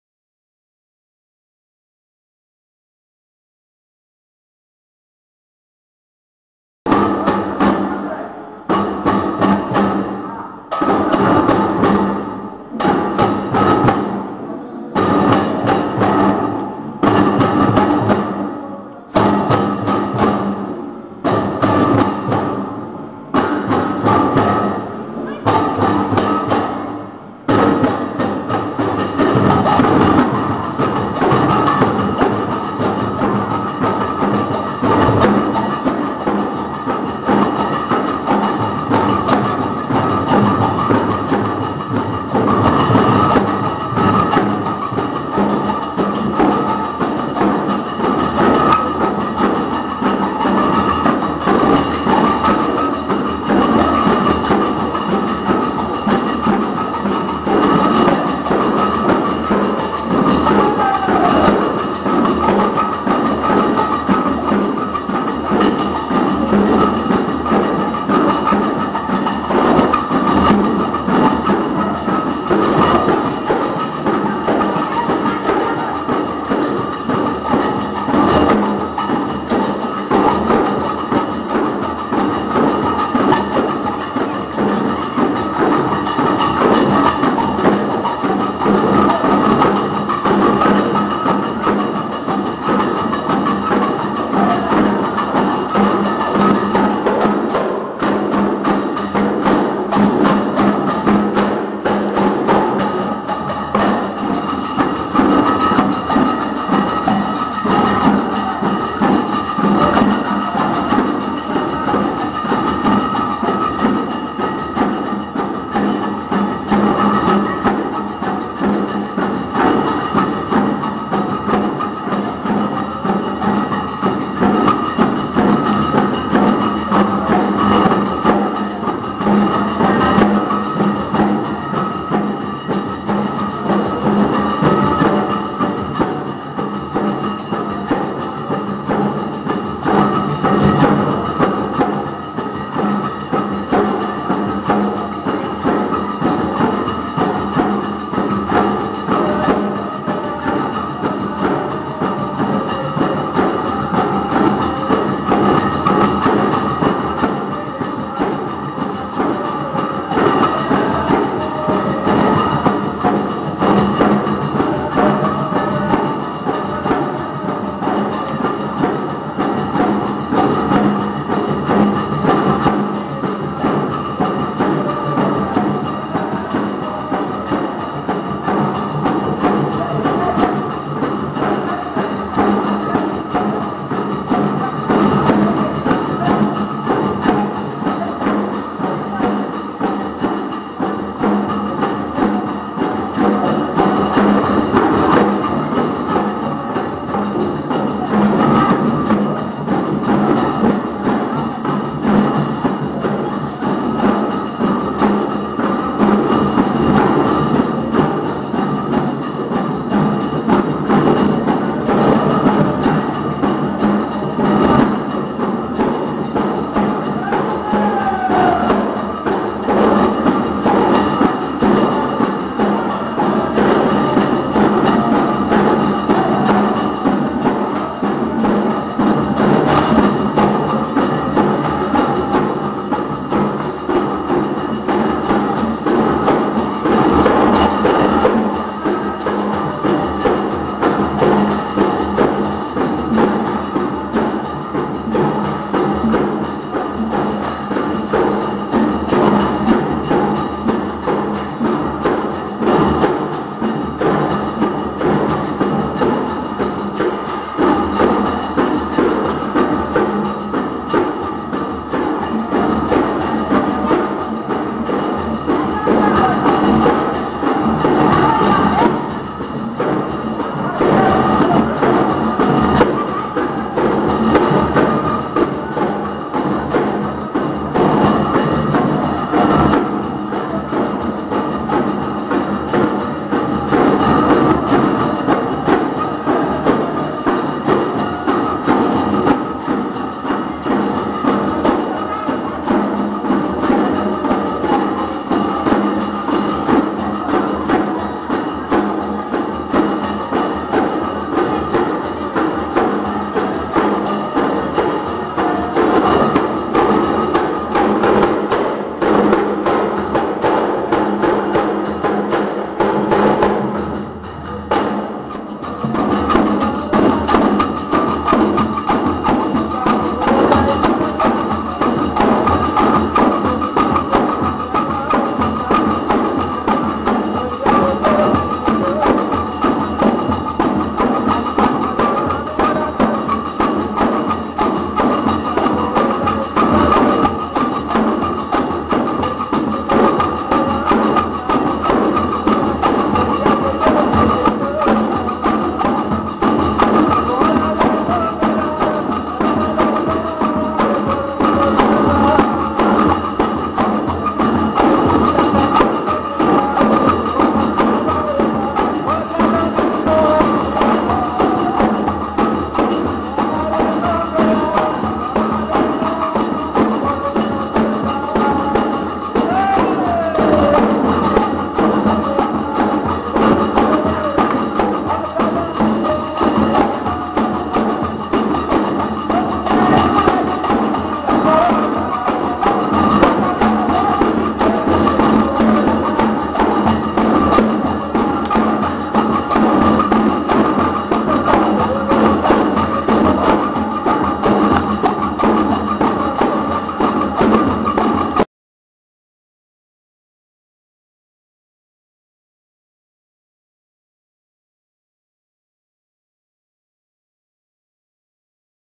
Niz Kala Kendra
Niz Kala Kendra Presenting Goan Dance with Mridula Shaha